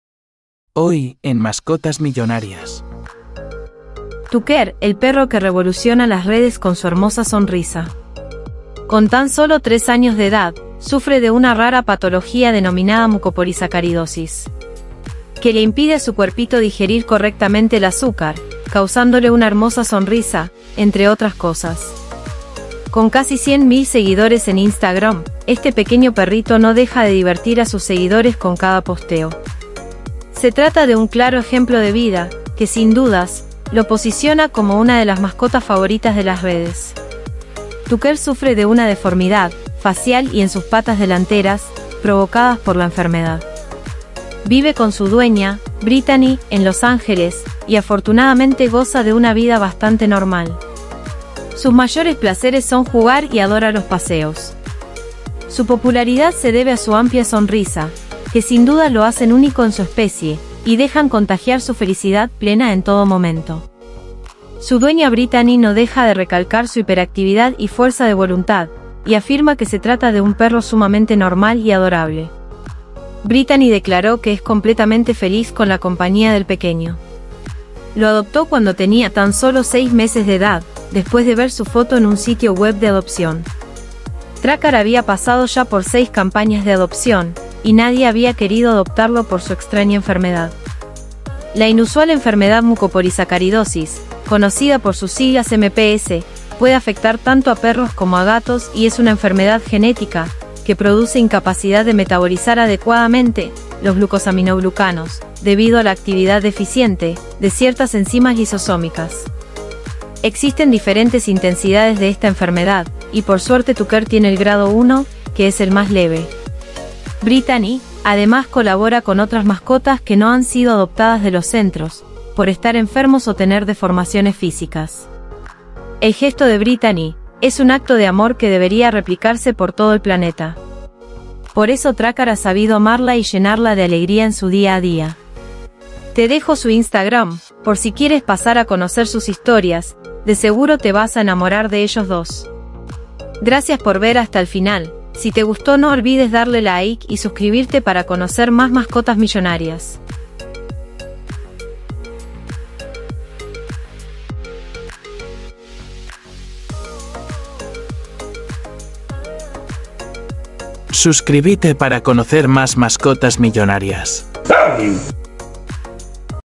TUCKER el perro SONRIENTE que te CONQUISTARA! un EJEMPLO de VIDA y LUCHA
Conoce a TUCKER, un ejemplo de vida y lucha que supera cualquier enfermedad!.